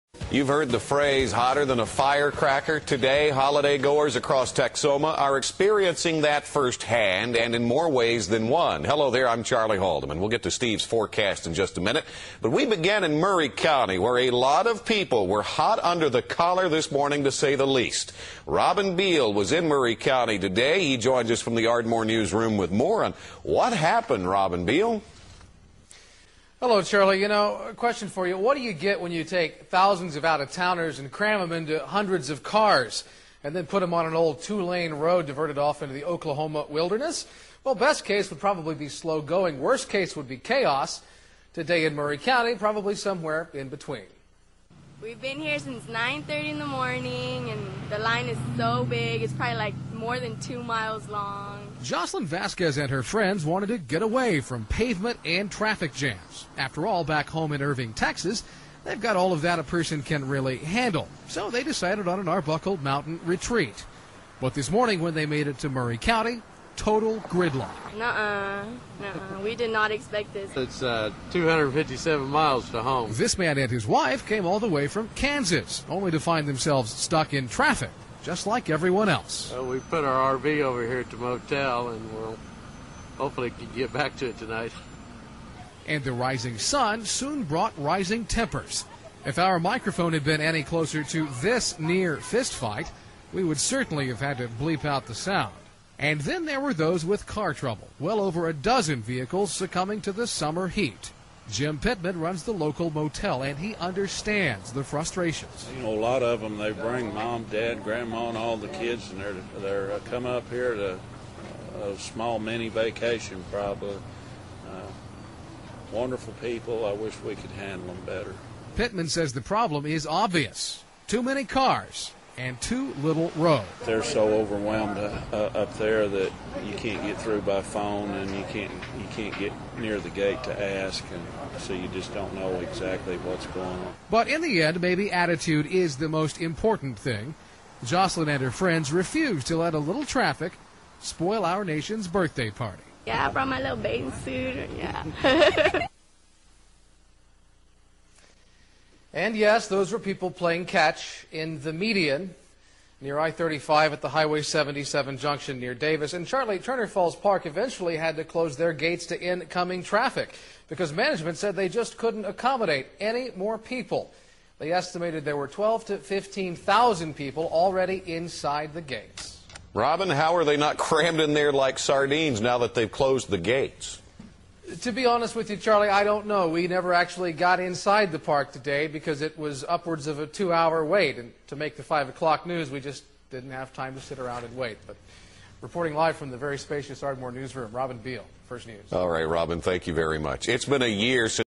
RB live OVERCROWDED VACATION 7/5/08
RB reports that too many people are trying to do the same thing.